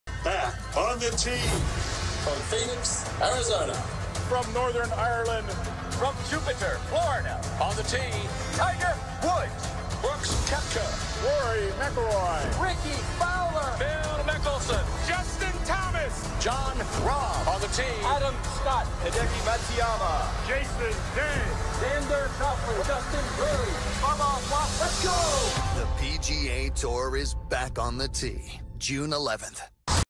PGA tour promo (05.28.20)
We are doing something different today we are showing you promos in Canada and USA from commercials